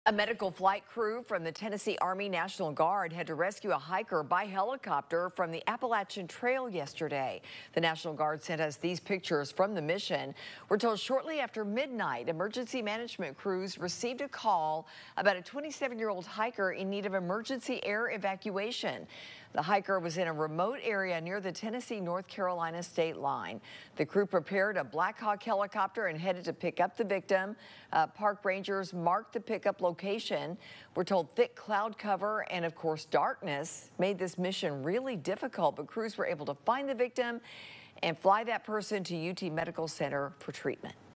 TN National Guard rescues hiker along the Appalachian Trail
A Tennessee National Guard flight crew rescued a hiker from the Appalachian Trail in the early morning hours of April 5.